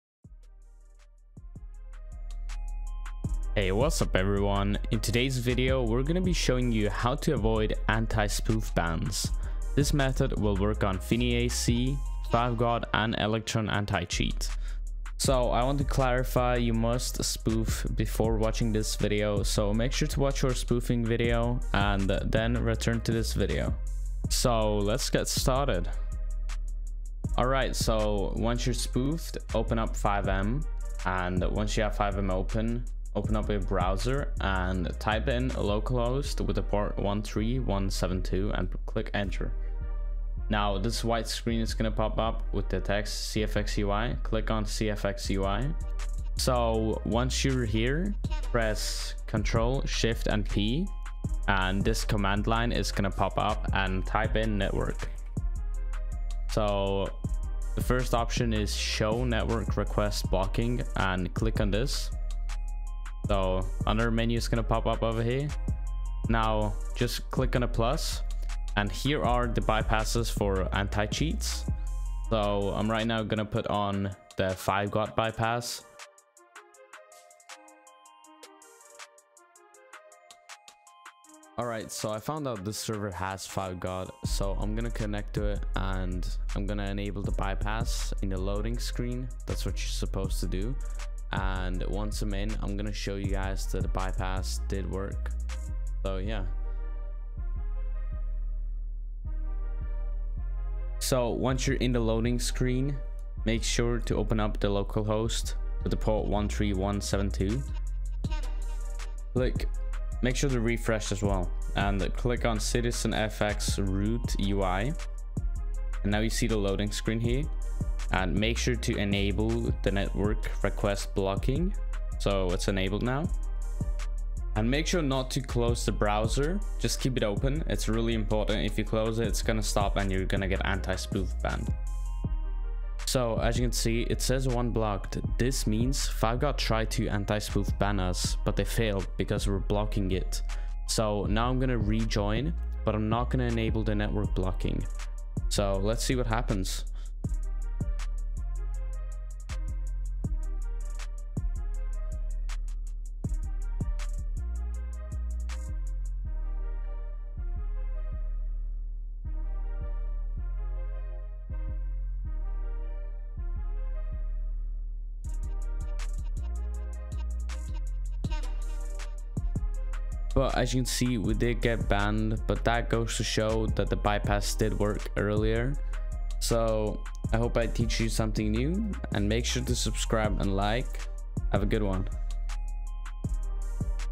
How to bypass server anti spoof on FiveM!
In this video, we'll walk you through techniques to bypass anti-spoof measures. Be sure to check out our earlier video on our privacy protection tool!